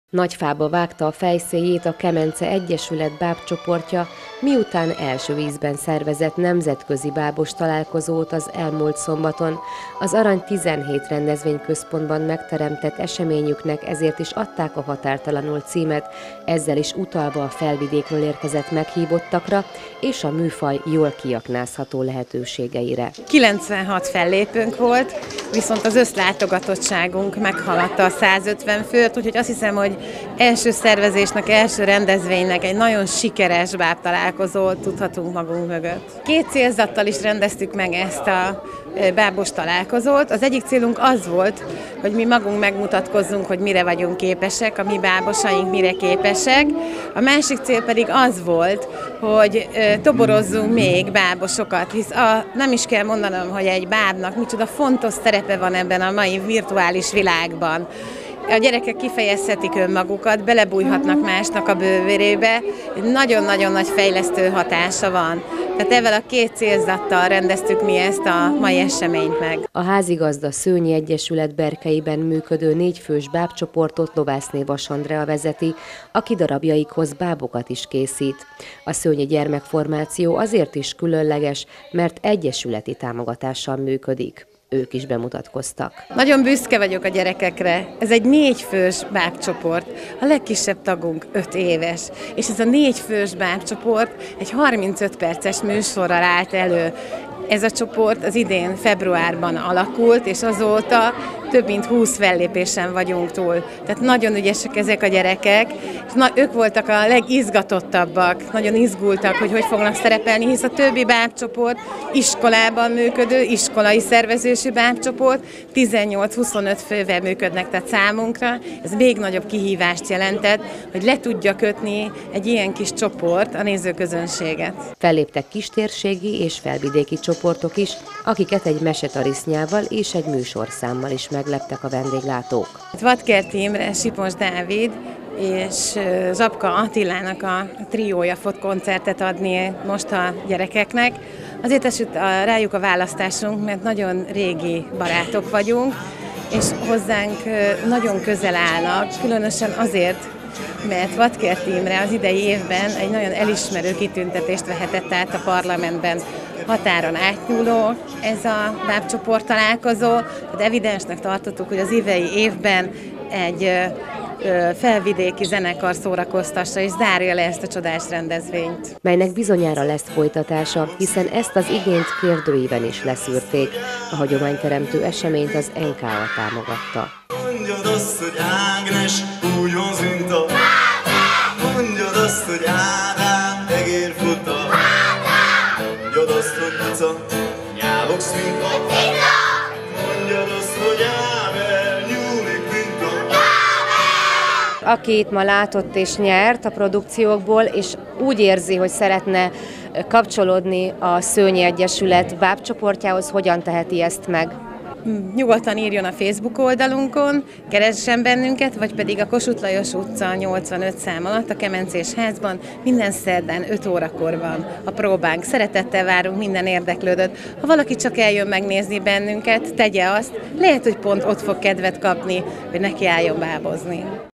Bábszínház az élet
Első alkalommal, de hagyományteremtő céllal szervezte meg a Kemence Egyesület a Határtalanul nemzetközi bábos találkozót.